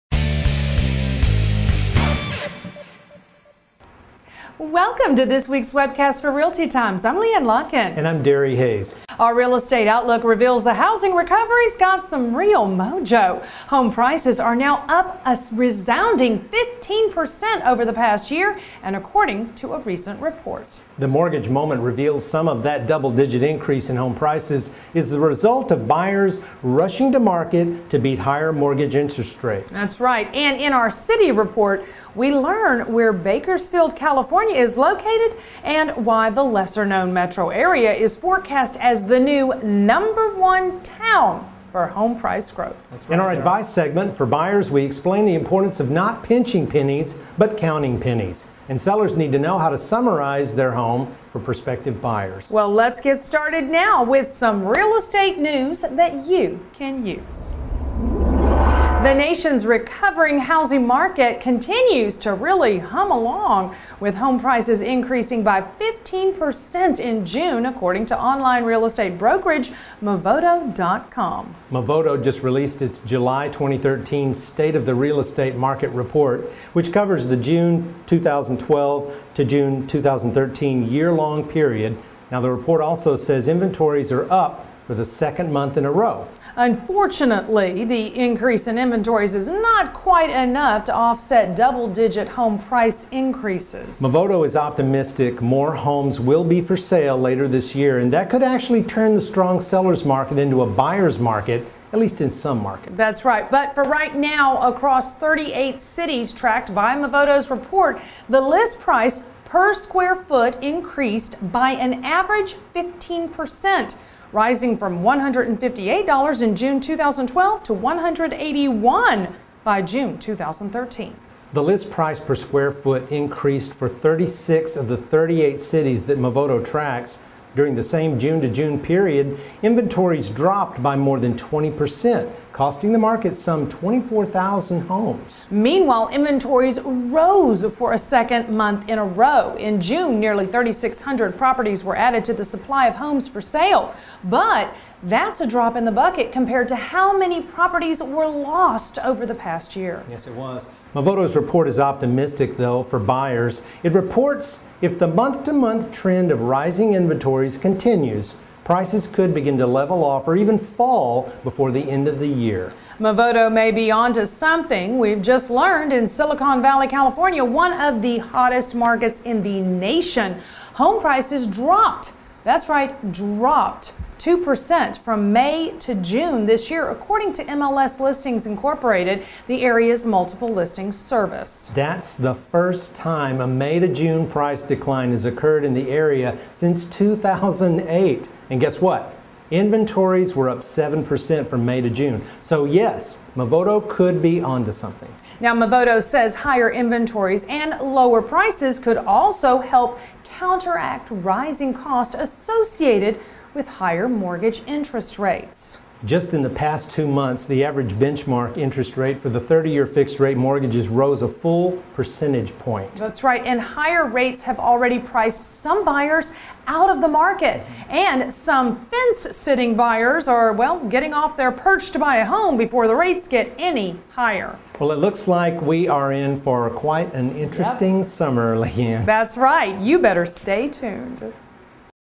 Redfin: Home Prices Soar 15% In June
The nation's recovering housing market continues to really hum along, with home prices increasing by 15 percent in June, according to online real estate brokerage Movoto.com.